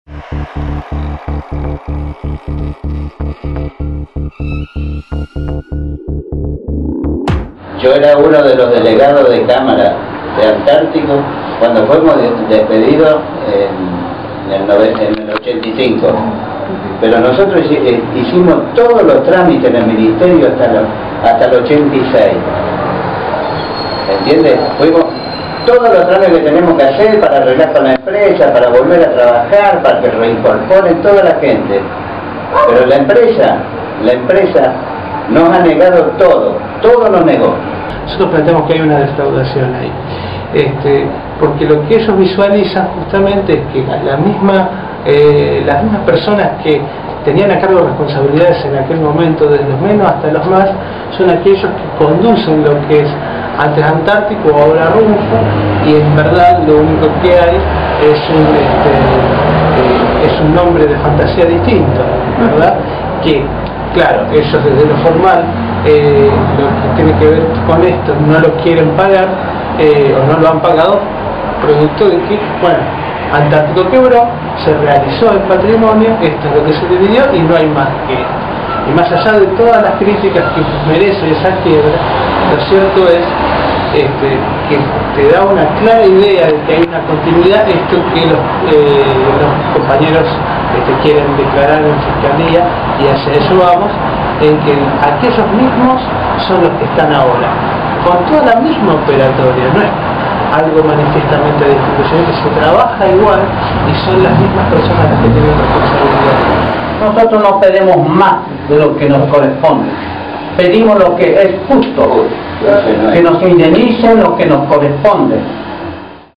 TRABAJADORES DE LA EX ANTÁRTICO
Exigen que el Ministerio de Trabajo los reciba
Los trabajadores despedidos del ex frigorífico Antártico, ubicado en Virrey del Pino, siguen reclamando por el pago de las indemnizaciones que la empresa les niega desde hace años. Piden que el Ministerio de Trabajo los reciba para que el conflicto pueda resolverse "con un final feliz".